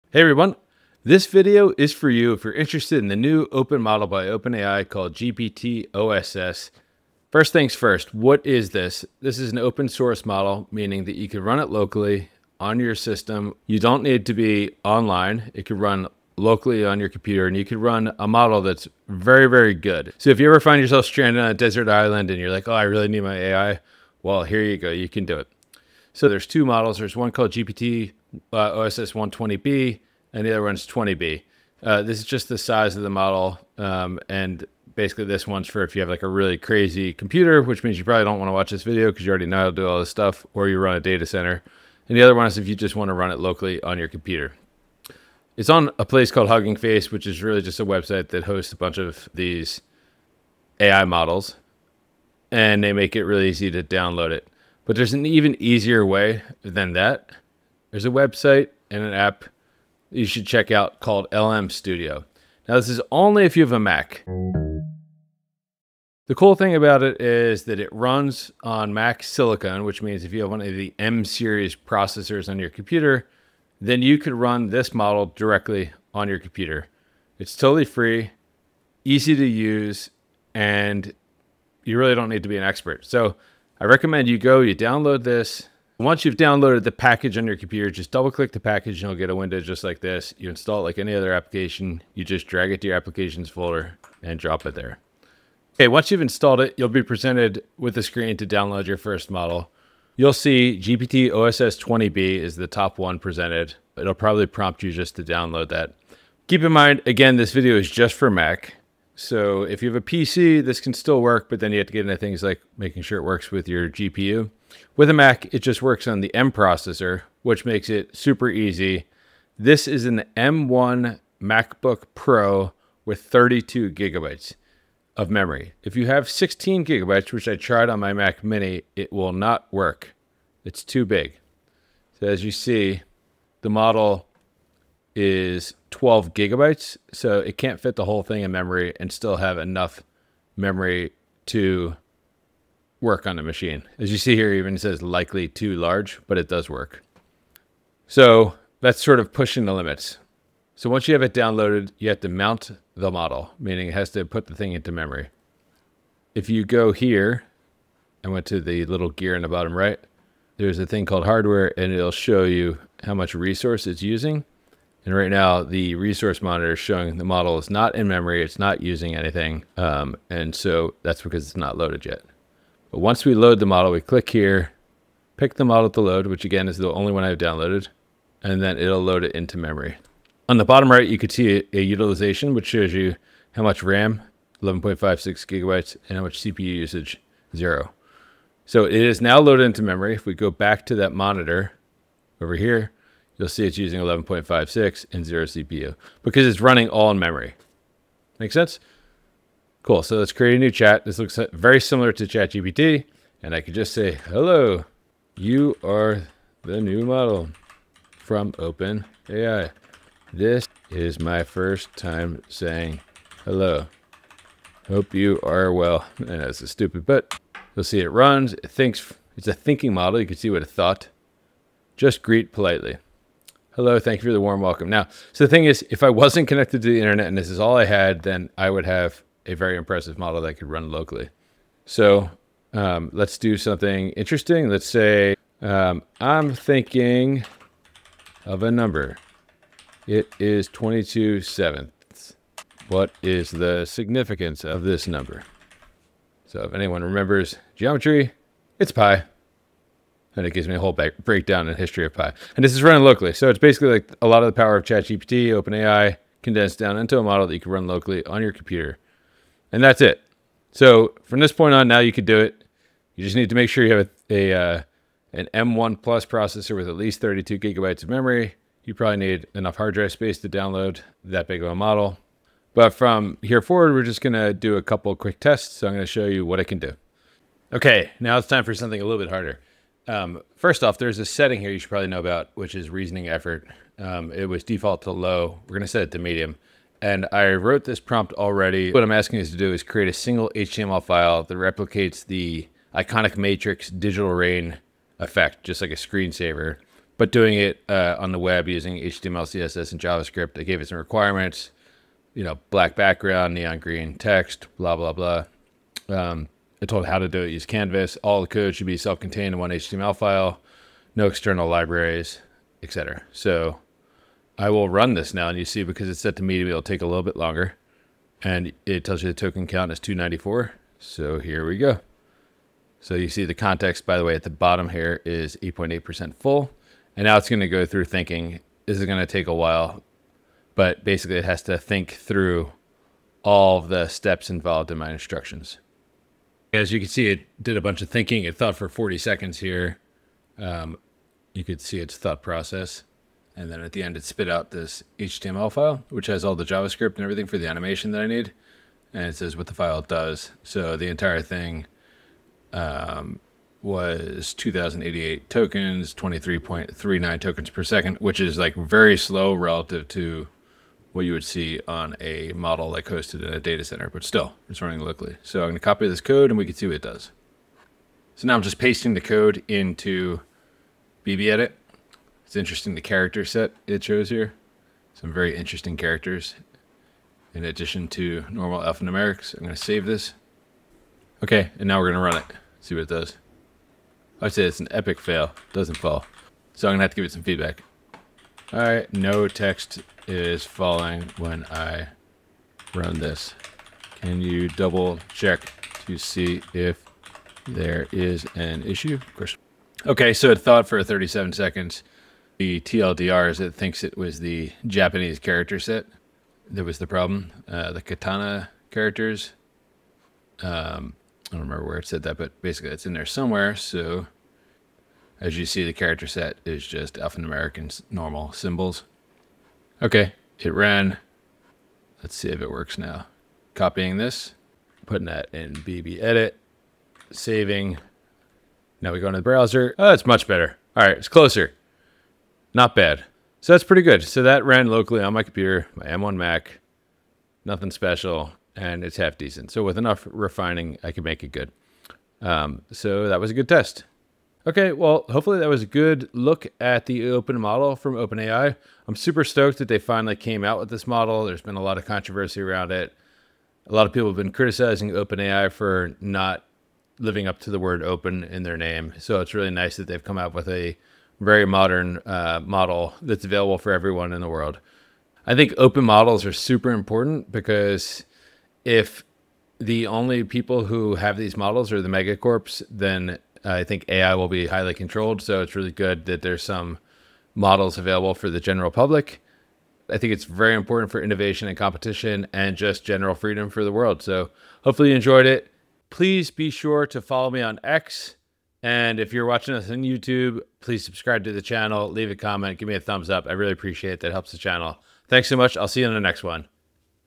Beginner’s Guide: Run OpenAI GPT-OSS 20B Locally on Your Mac (Step-by-Step)
Learn how to run the new OpenAI GPT-OSS 20B model entirely offline on your Mac — no internet required. In this beginner-friendly tutorial, I walk you through downloading, installing, and running GPT-OSS locally using LM Studio, plus we test it with some fun prompts.

You’ll learn:

* What GPT-OSS is and why it matters
* How to install LM Studio on your Mac
* Hardware requirements (M1/M2 + memory)
* How to load and run GPT-OSS 20B
* Real examples, including generating HTML/JavaScript code locally

Requirements:

* Mac with M1/M2/M3 processor
* 32GB RAM recommended

👥 Are you a founder?

Chapters
00:00 – What is GPT-OSS?
00:31 – GPT-OSS model sizes explained
01:00 – Why LM Studio makes it easy on Mac
01:17 – M-series processor requirement
01:35 – Installing LM Studio
01:46 – Downloading your first model
02:46 – Hardware requirements & RAM limits
03:30 – Loading GPT-OSS 20B into memory
03:51 – Checking resource usage
04:28 – Running first local prompt
04:54 – Local performance & limitations
05:00 – Setting “Reasoning Effort” to Medium
05:27 – Prompt: Matrix digital rain effect (HTML/CSS/JS)
06:00 – First attempt & debug
06:39 – Fixing the code & re-running
07:08 – Successful animation output
07:24 – Final thoughts on open models